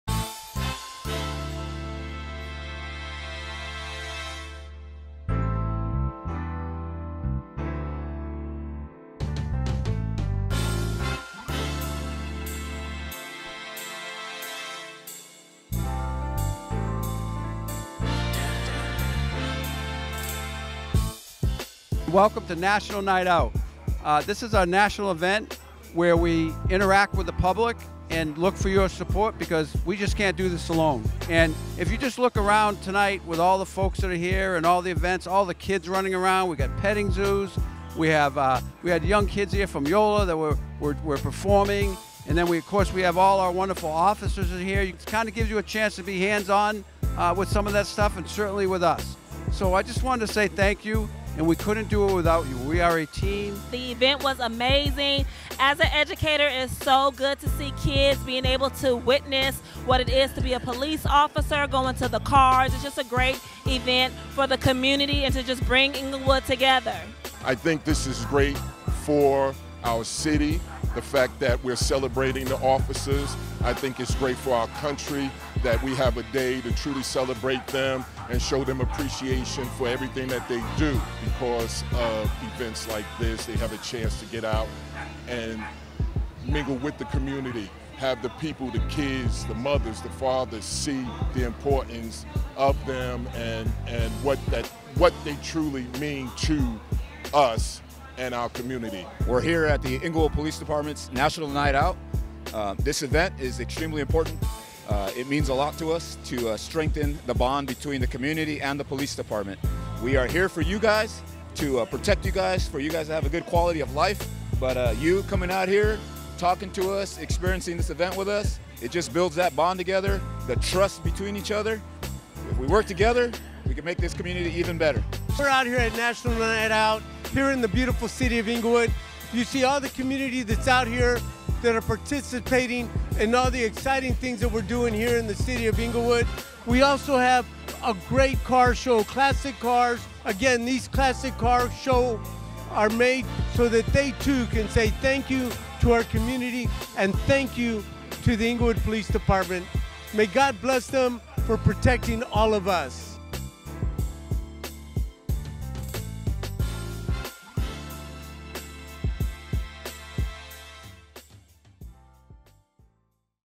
2024 City of Inglewood National Night Out
This year’s National Night Out was a tremendous success, uniting neighbors and law enforcement to strengthen our sense of community and partnership. A big thank you to our men and women of the Inglewood Police Department, sponsors and community partners for making this event happen. Together, we’re building a safer and stronger Inglewood.